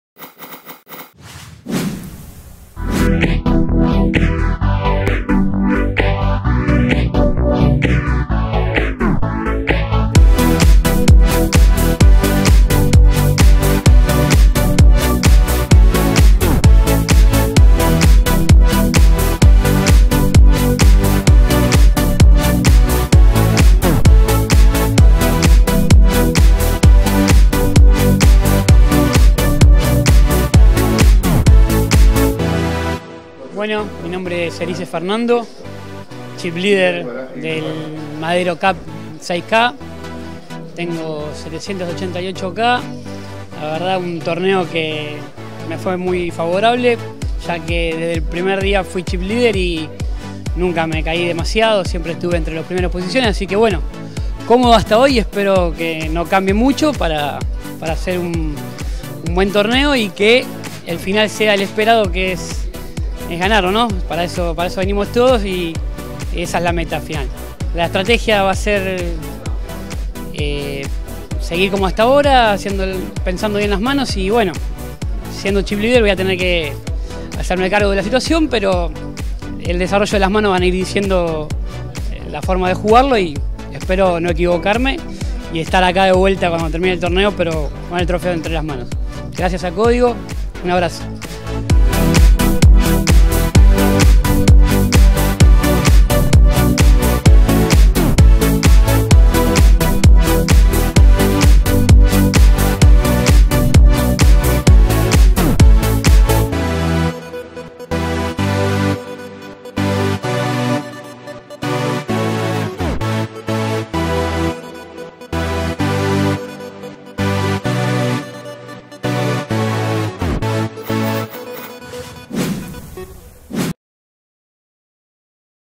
Madero Poker Summer Cup 6k - Inicio del Dia 3
Se rompe la burbuja en el día 2 de la Madero Poker Summer Cup.